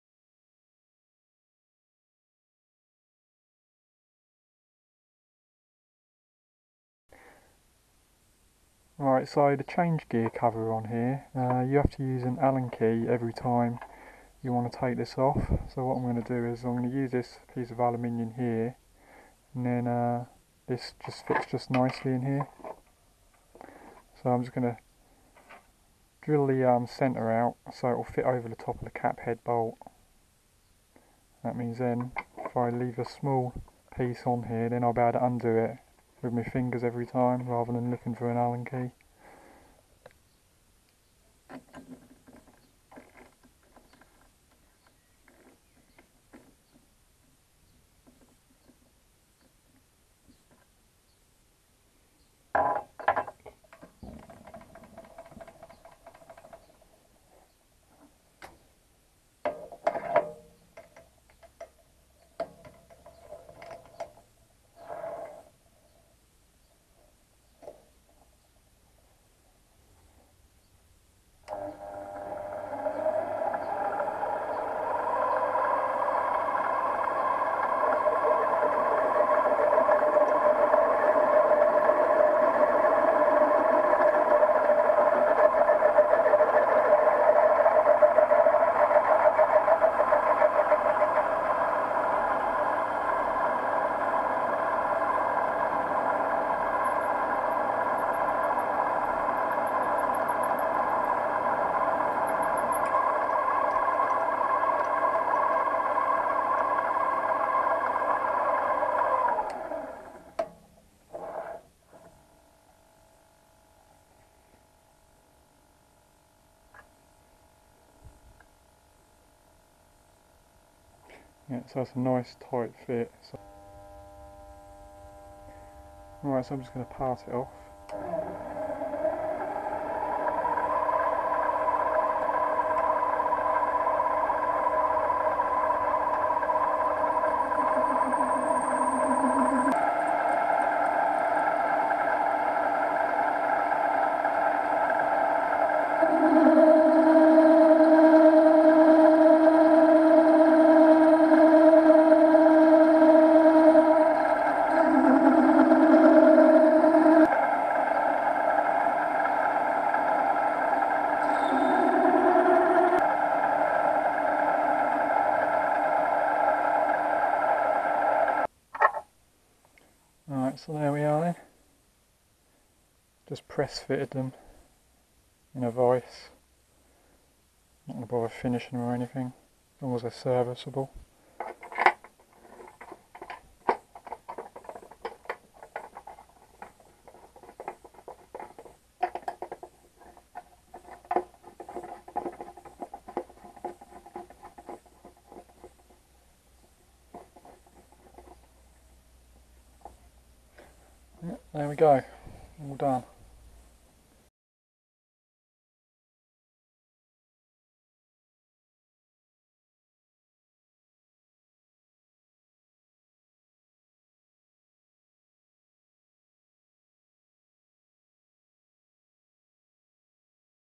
Diy Thumb screws change gear cover c3 mini lathe
my Warco sieg 7x14 howtoDiy Thumb screws change gear cover
from old Hard drive video archive.

QUICK CHANGE TOOL POST